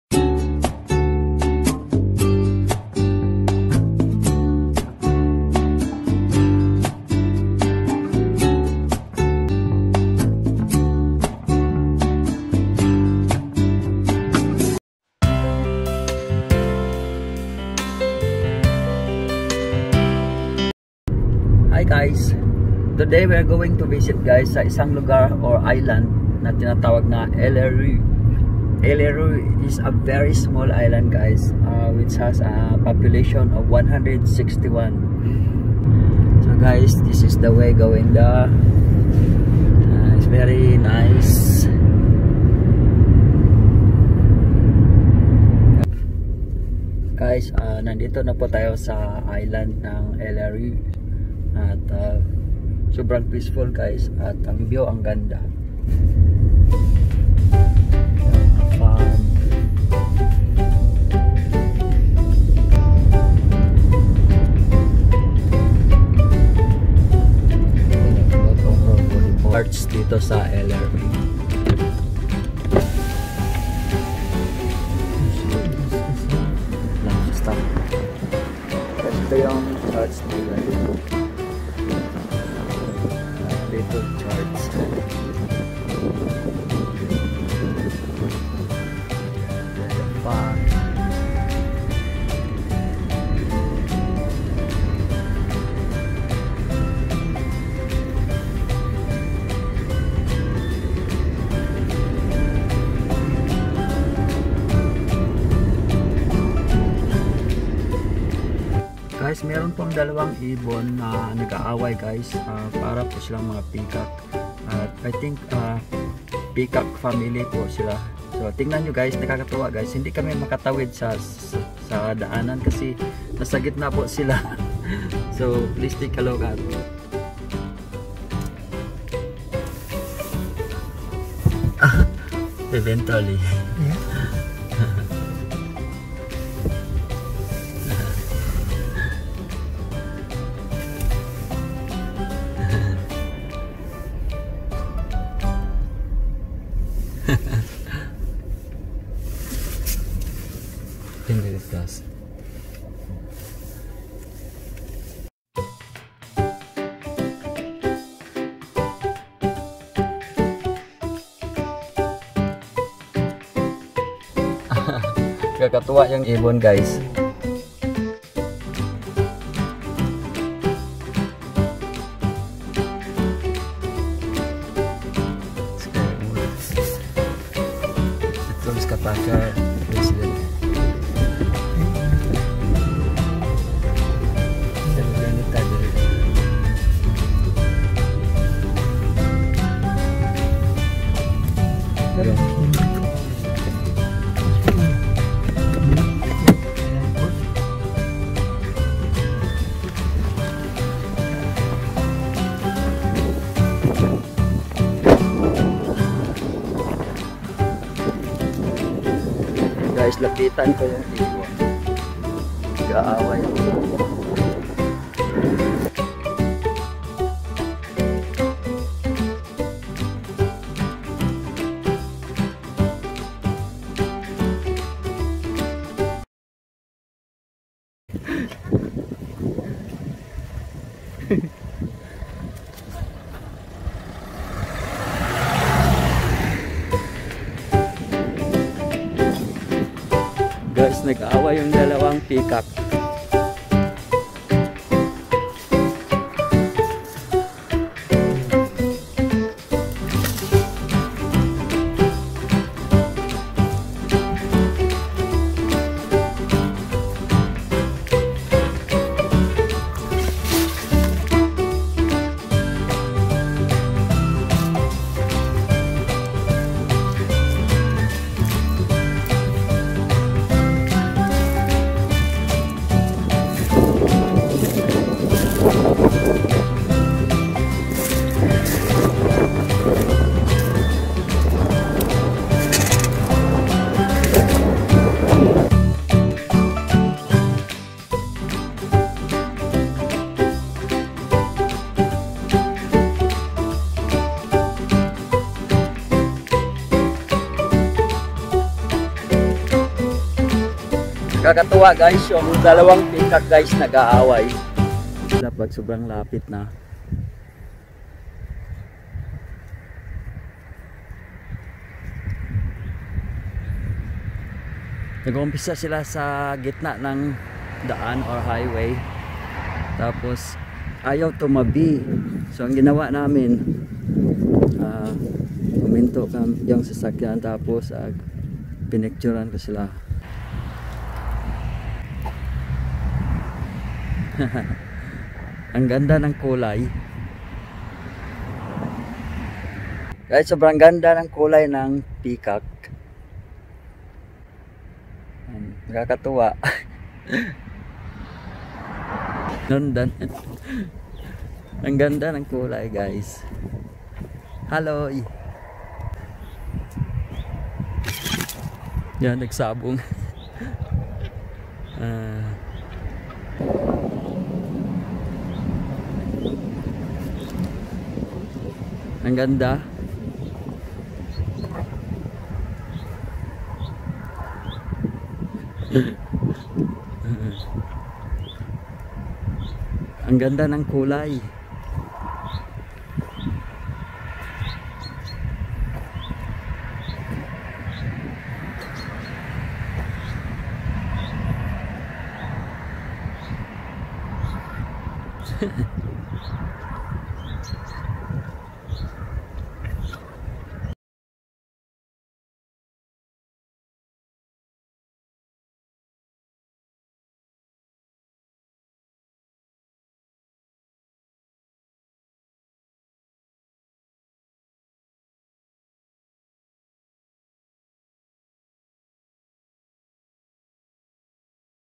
PHEASANTS FIGHT IN THE ISLAND OF ALRØ, By: Jeff Artajo
The 2 male pheasants are fighting for a female pheasant.
While on the roadtrip to the beautiful island "Alrø", suddenly there was 2 pheasants fighting in the middle of the road, so i decided to make a video of this 2 beautiful pheasants...